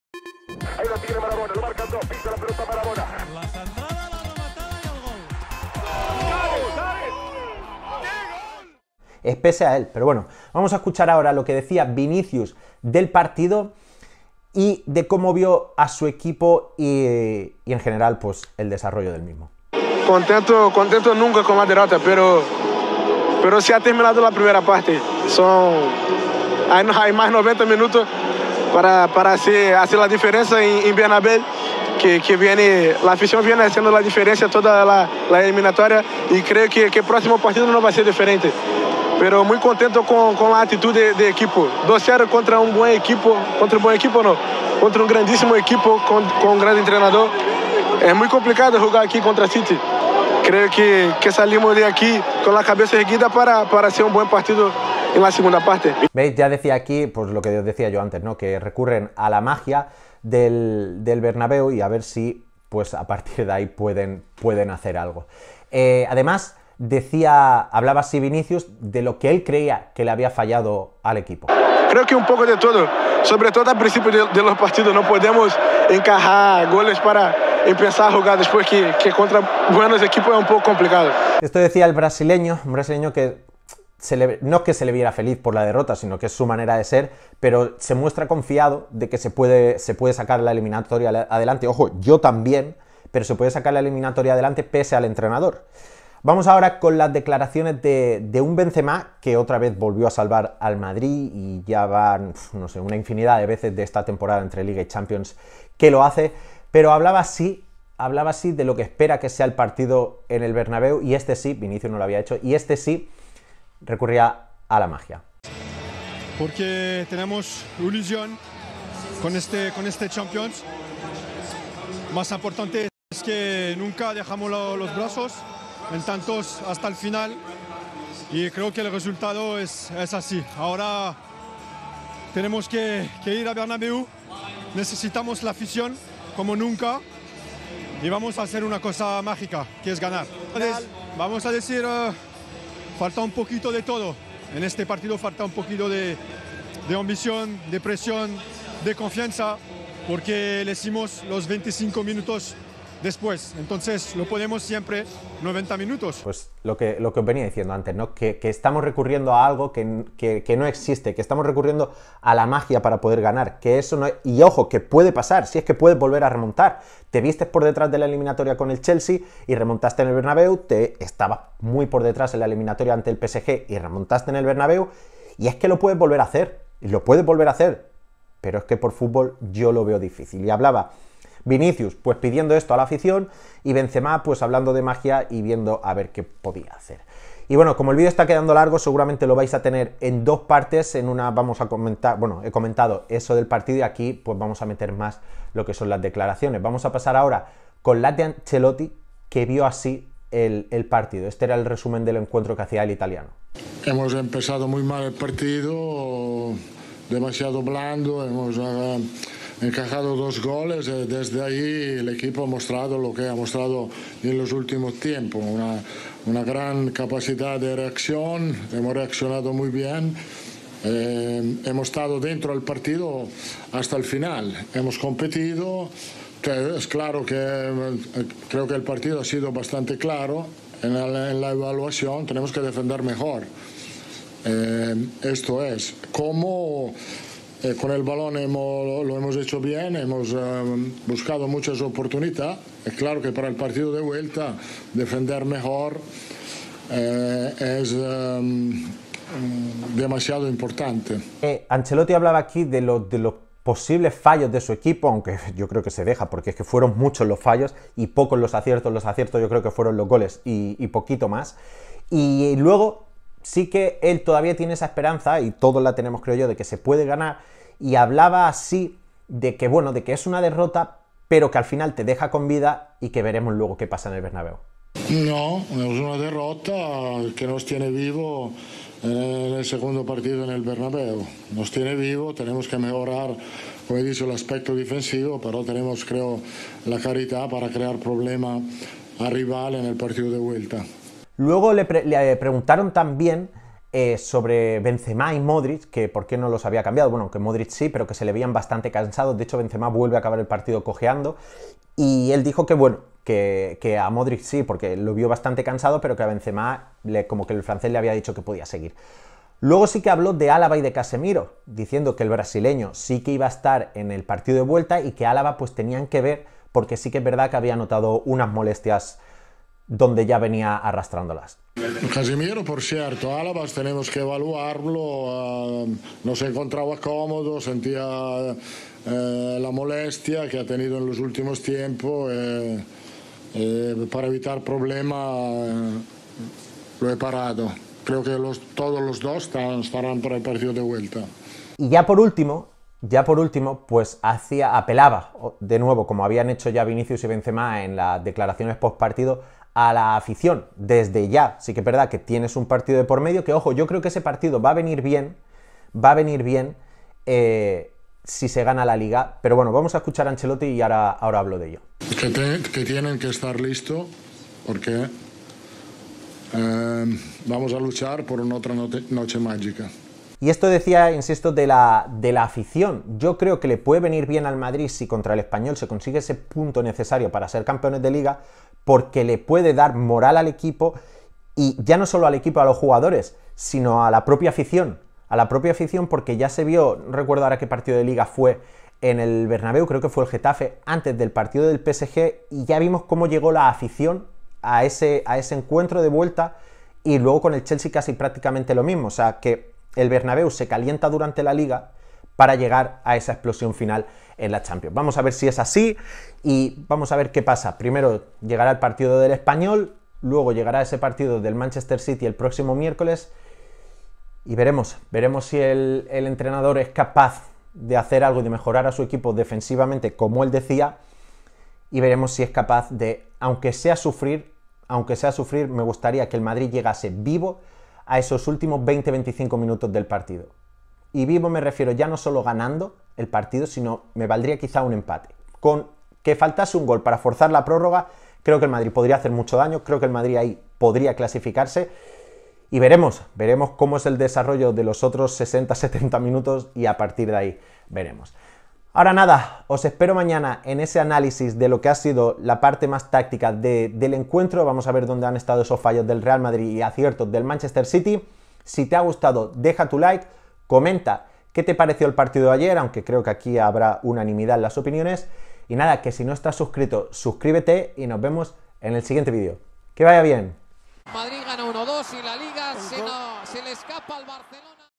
🎙ANCELOTTI, VINICIUS y BENZEMA: ¿QUÉ hacer para GANAR? | 🤔✅ Las CLAVES para el SANTIAGO BERNABEU 🏟
🟣⚪️ Estas fueron las declaraciones más importantes tras la derrota ante el Manchester City. Entrenador y los dos autores de los goles COINCIDEN en qué debe hacer el equipo para remontar.

✅ Ancelotti, Vinicio y Benzema, además, pidieron desde ya el apoyo del Bernabéu para hacer lo que mejor saben hacer: REMONTAR EN CHAMPIONS LEAGUE.